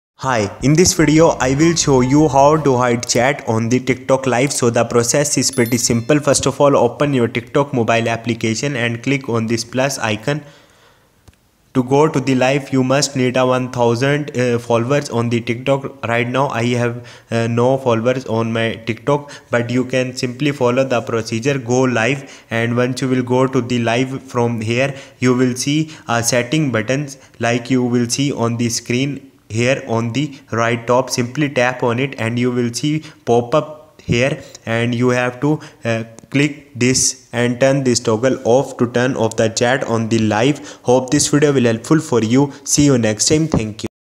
How To Hide Chat On TikTok Live
Learn How To Hide Chat On TikTok Live

In this video I will show you how to hide chats on TikTok live. This will be a short guide that shows you exactly how to hide TikTok live chat.